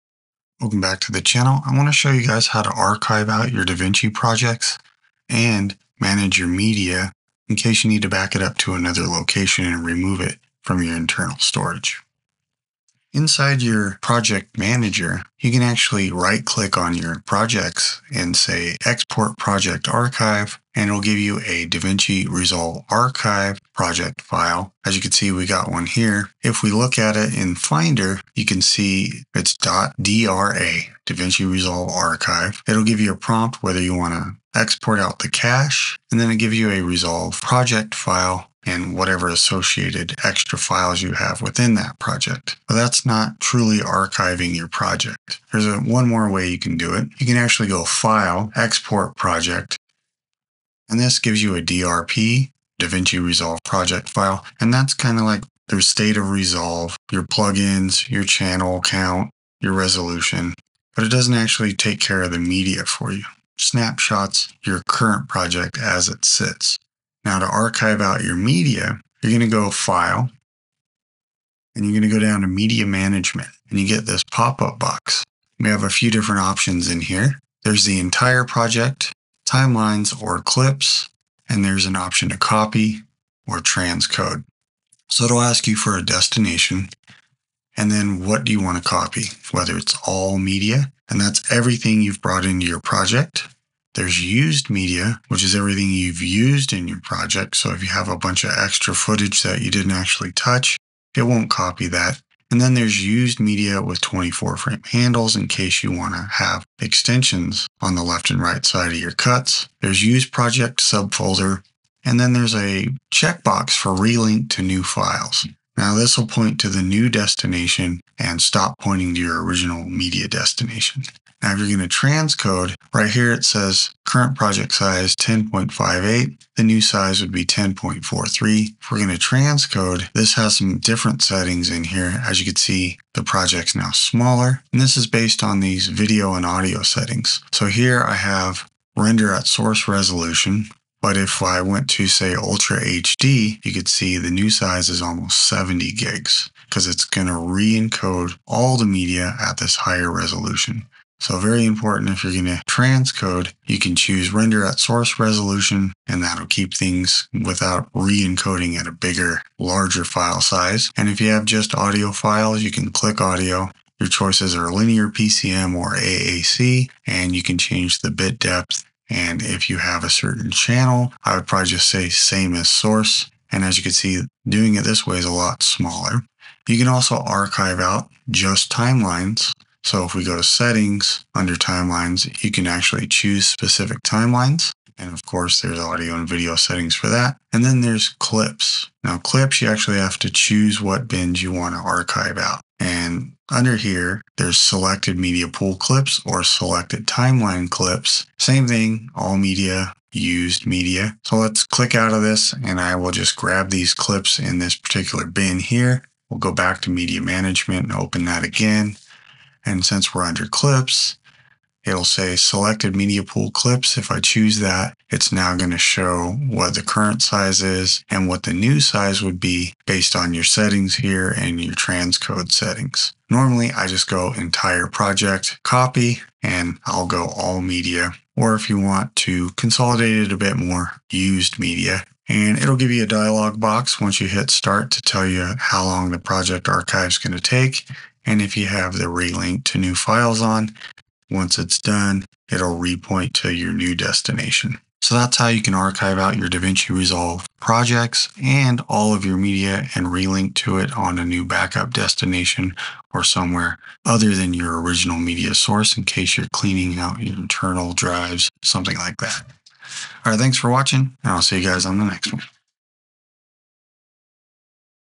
How to Properly Archive DaVinci Resolve Projects, Media & Relinking
Backing up your DaVinci Resolve projects the right way is essential—especially if you’re offloading internal storage or moving to an external drive. In this video, I’ll walk you through how to archive your project and media using both the Project Archive and Media Management tools inside Resolve.

🔍 What you'll learn:

The difference between .dra (Project Archive) and .drp (Project File)

How to export your full project archive with media, cache, and project settings

How to use Media Management to copy or transcode

What the options "All Media", "Used Media", and "Used Media with Handles" really mean

How to relink media to a new location automatically

Tips for keeping your archive size efficient

How to archive individual timelines or bins if needed

Whether you're backing up to cloud storage, external SSDs, or prepping for long-term project storage, this video has you covered.

00:00 Intro
00:12 Project Archive from Project Manager
00:45 Project Export
01:06 Media Archive
02:01 Transcoding
02:56 Timeline Archives

💬 Drop a comment if you’ve got questions about archiving or want to request a future tutorial!